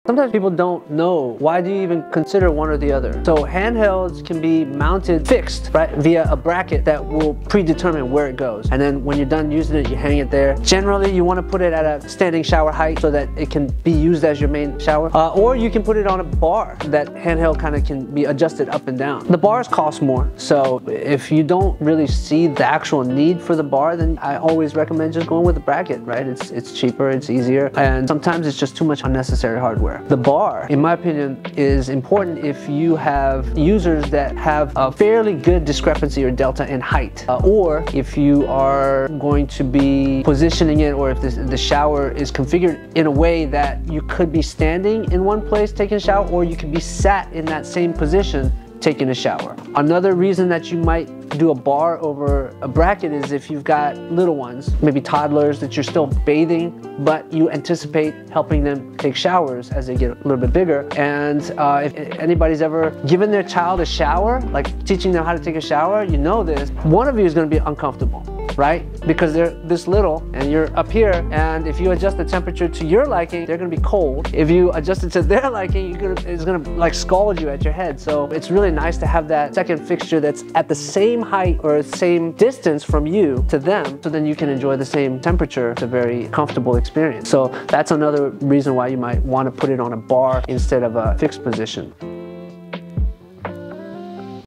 Bracket vs. Bar: Which is Best for Your Remodel? Calvin's Expert Advice!
Join Calvin as he dives into the debate between brackets and bars for your remodeling project. Discover the pros and cons of each option and make the right choice for your home! Don't miss out—watch now for valuable insights.

Learn more and make an informed decision for your remodel journey!